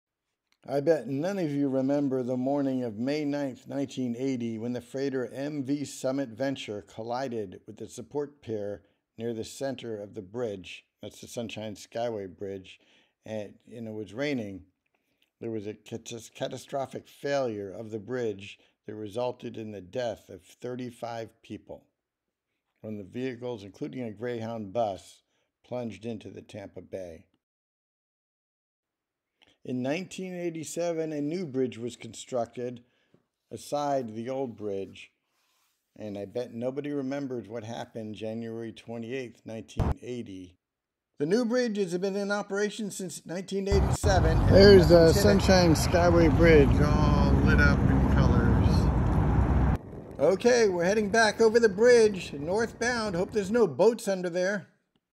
Tragedy of the Summit Venture: The Collapse of the Sunshine Skyway Bridge
On May 9, 1980, the Summit Venture cargo ship collided with the Sunshine Skyway Bridge in Tampa Bay, causing a massive collapse that claimed the lives of 35 people. In the aftermath of the disaster, a new bridge was built to replace the destroyed span. In this video, we explore a short history of the Summit Venture tragedy and the construction of the new Sunshine Skyway Bridge, completed in 1987. SunshineSkywayBridge SummitVenture TampaBay disaster history construction engineering infrastructure Florida"